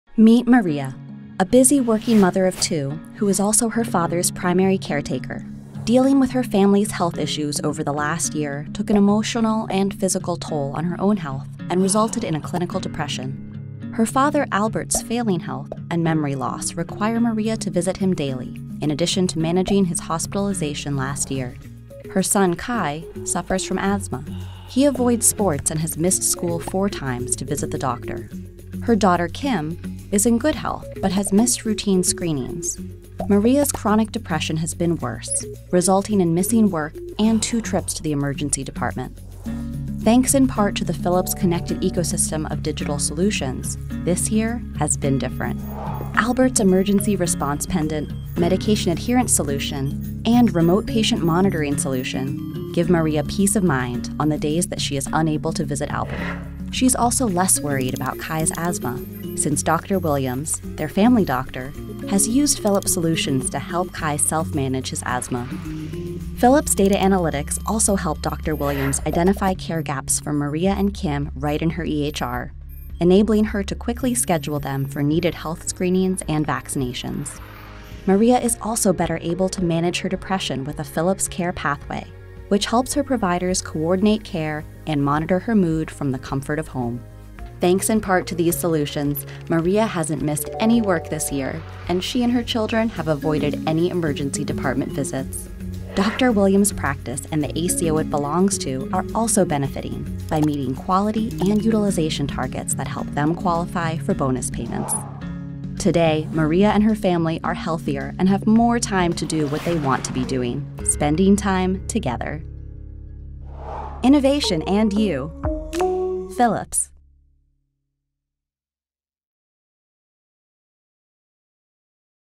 One caregiver’s collaborative healthcare experience
Patients aren’t the only ones impacted by failing health. Their caregivers are too. This video shows how Philips solutions can help people like Maria, whose own health is worsening as she cares for her aging father and two children while holding down a full-time job.

Philips is seamlessly connecting people, data and technology to help people live healthier lives, and better empower the providers who care for us.